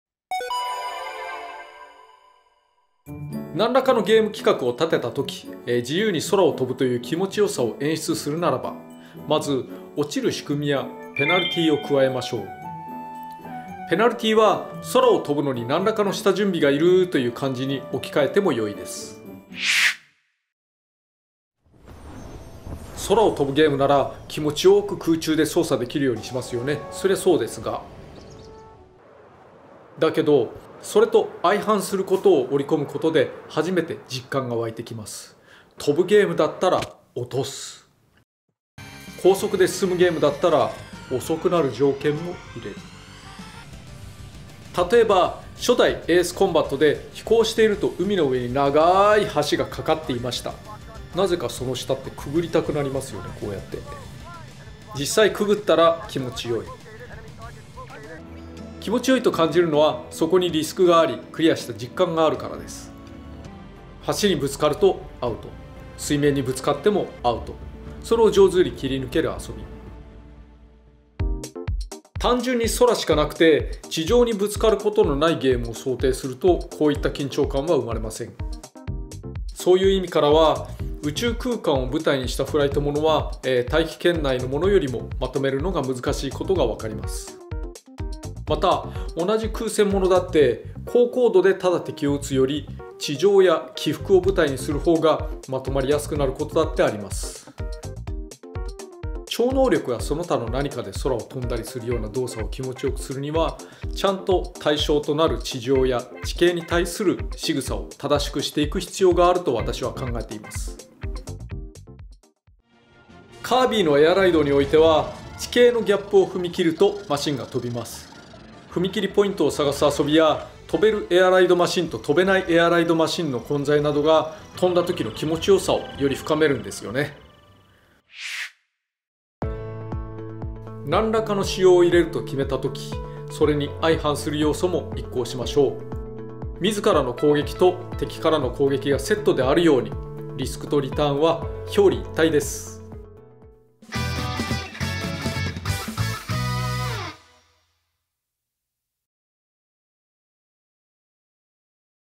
Falling Sells the Feeling of Flight [Game Essence]
If you want your players to truly experience the thrill of flight, you need to also include the not-so-thrilling stuff like crashing and falling!
Opposing elements, when implemented correctly, will heighten and amplify the best parts of your game.